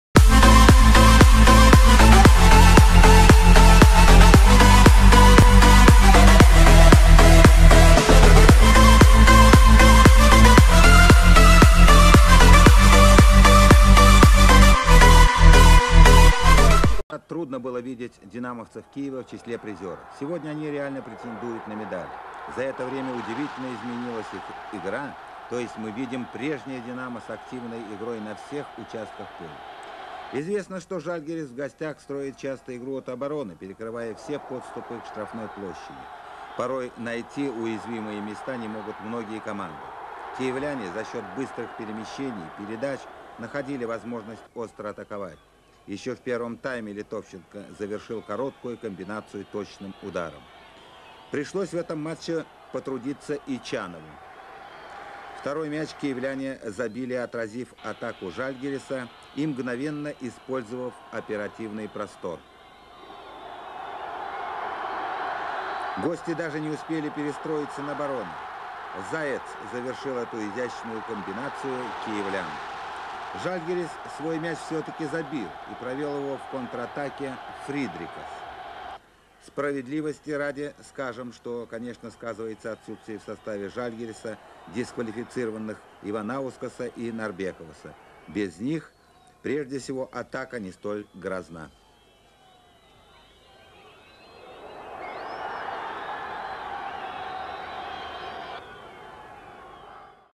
22.09.1989 "Динамо" Київ - "Жальгіріс" Вільнюс 2:1
Чемпіонат СРСР 1989 року.
Використано відео YouTube Андрей Попов